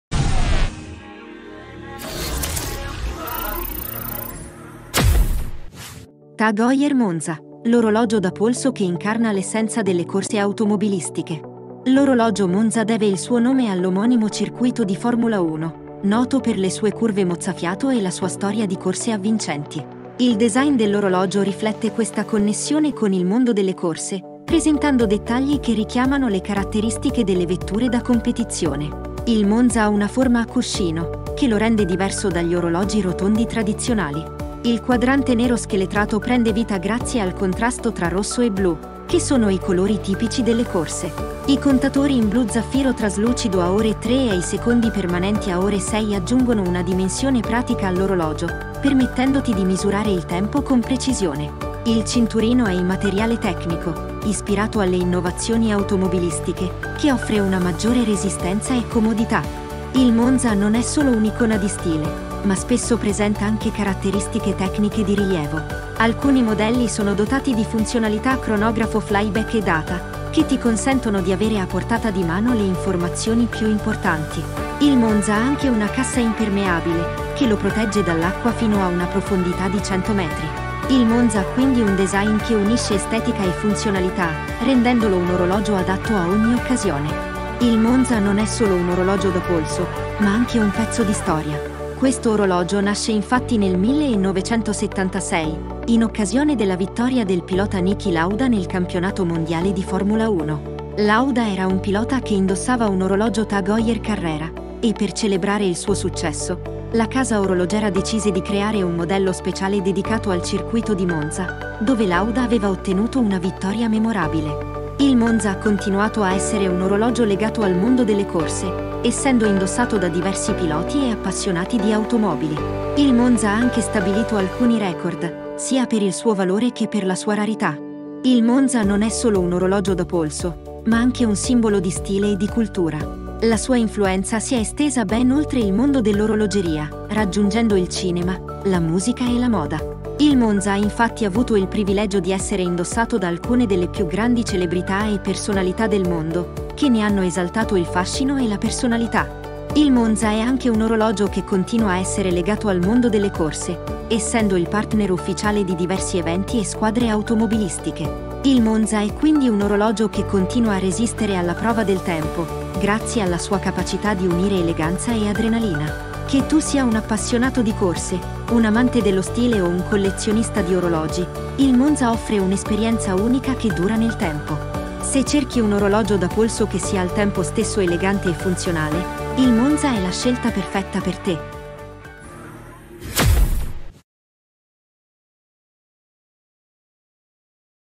TAG HEUER MONZA: l'orologio da polso che incarna l'essenza delle corse automobilistiche
Scopri le caratteristiche uniche del Monza, l’orologio da polso che riflette la connessione tra l’eleganza e l’adrenalina del mondo delle corse automobilistiche.